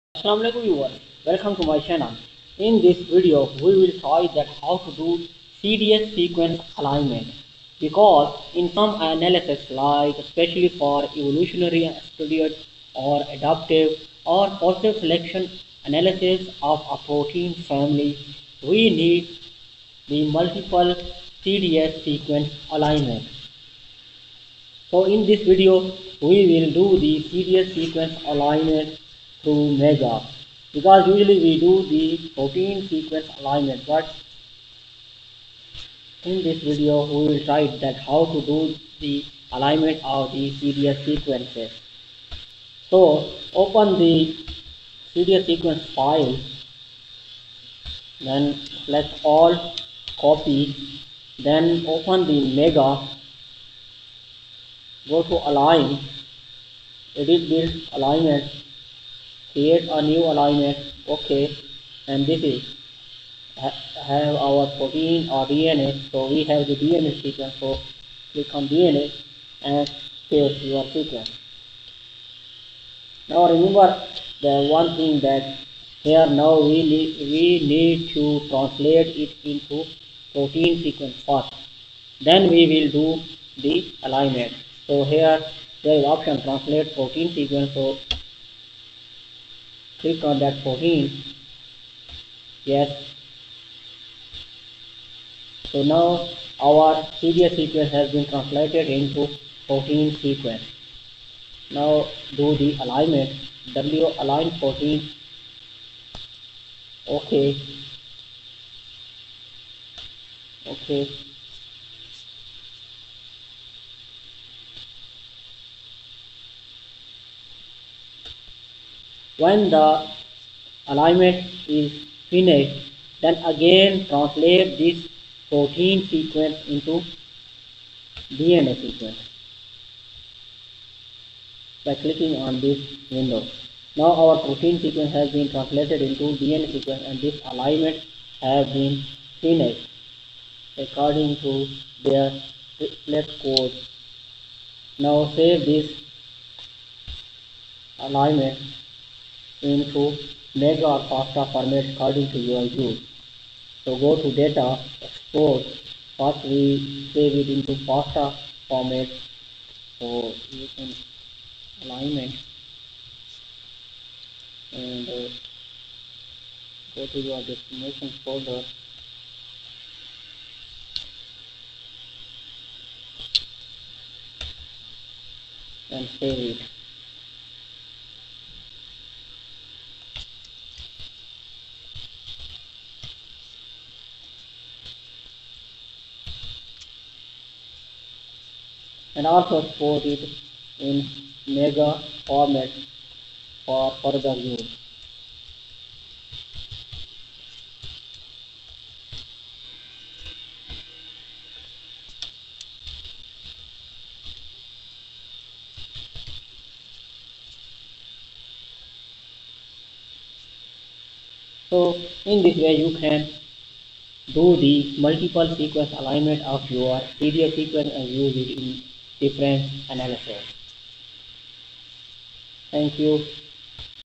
DNA sequence alignment | protein alignment | ClustalW MEGA7 | DNA to Protein alignment | Seq align
ClustalW is a general purpose DNA or protein multiple sequence alignment program for three or more sequences. For the alignment of two sequences please instead use our pairwise sequence alignment tools. Sequence alignment is the process of comparing and detecting similarities between biological sequences. What “similarities” are being detected will depend on the goals of the particular alignment process. Sequence alignment appears to be extremely useful in a number of bioinformatics applications. For example, the simplest way to compare two sequences of the same length is to calculate the number of matching symbols. The value that measures the degree of sequence similarity is called the alignment score of two sequences. The opposite value, corresponding to the level of dissimilarity between sequences, is usually referred to as the distance between sequences. The number of non-matching characters is called the Hamming distance.